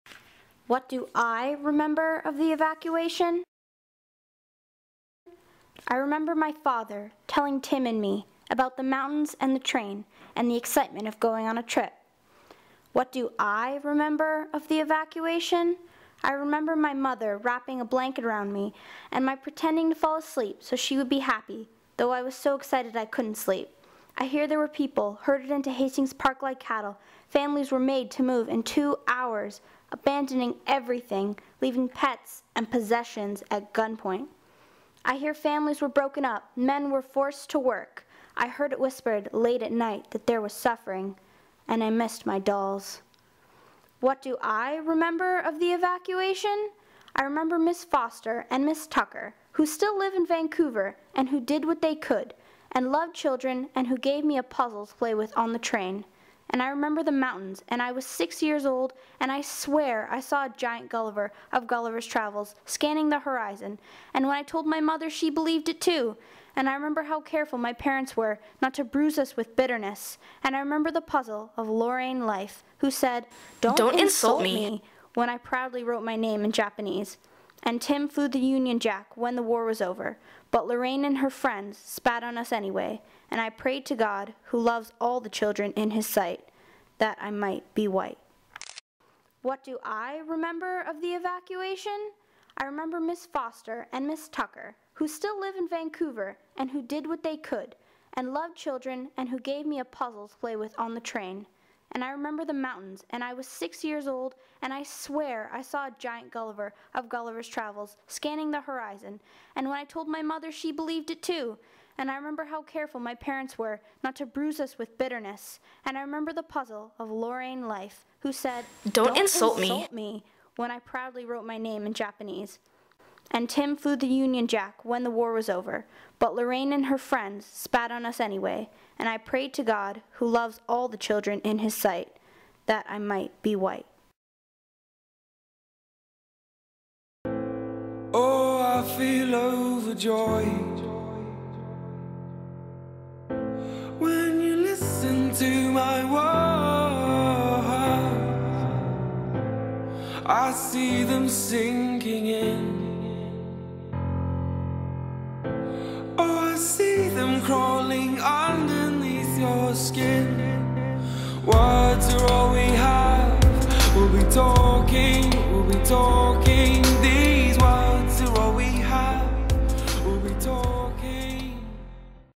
What Do I Remember of the Evacuation-Joy Kogawa
An interpretation of the poem What Do I Remember of the Evacuation by Joy Kogawa told in the perspective of a child.